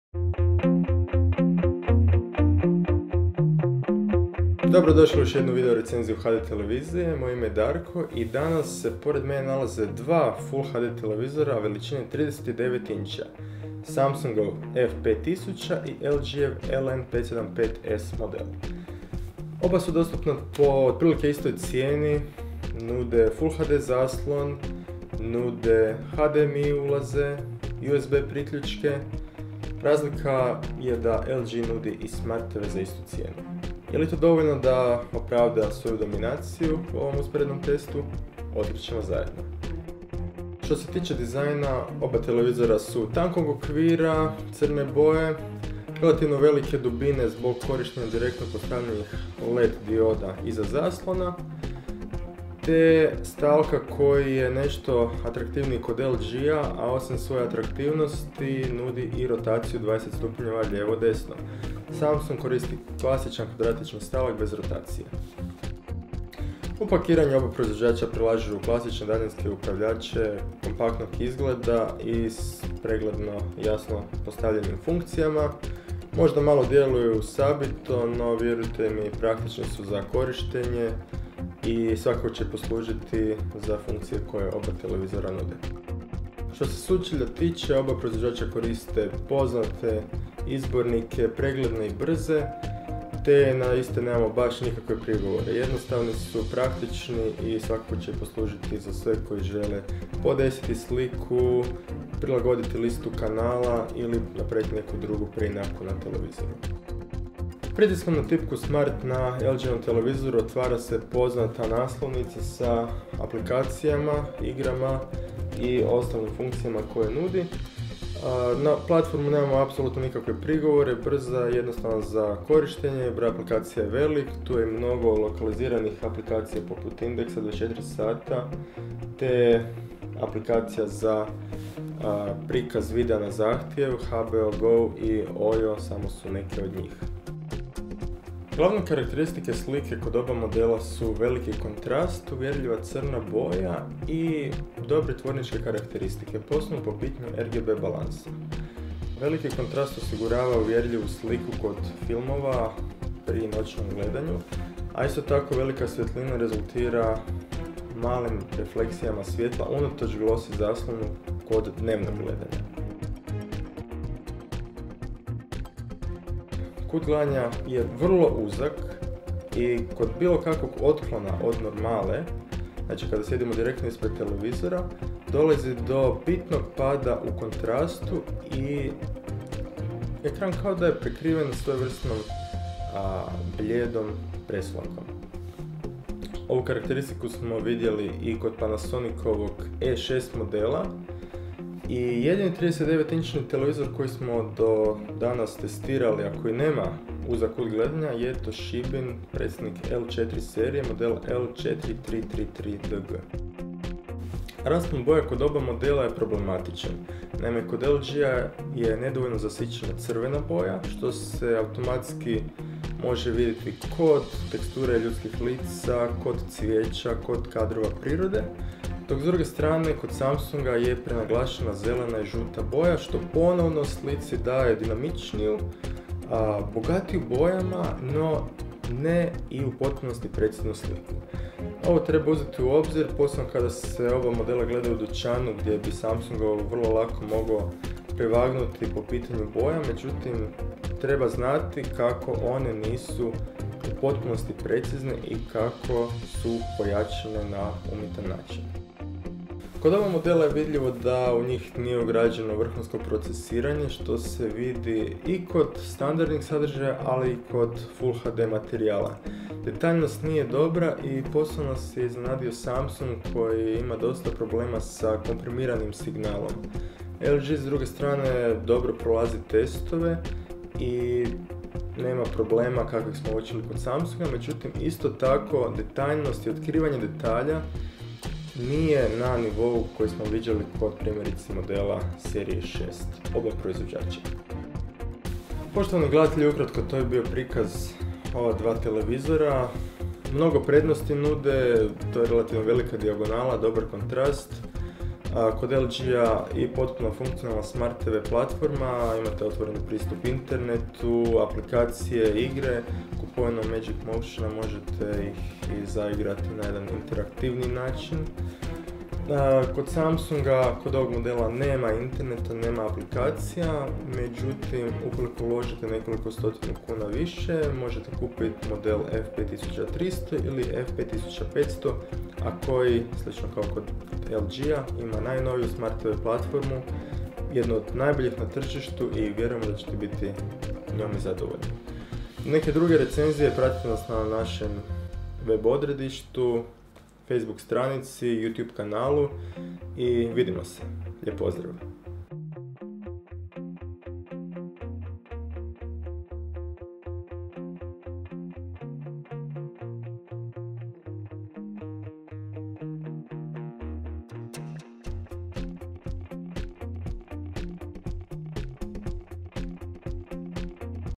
HDTV test: Samsung 39F5000 vs. LG 39LN575S
Video presentation of two entry level TVs from Samsung and LG, both available at the same market price. Main difference is that LG offers full Smart TV platform powered by dual core CPU, while Samsung only offers video/photo/music playback through USB connection. Full review with calibration details and many other information is available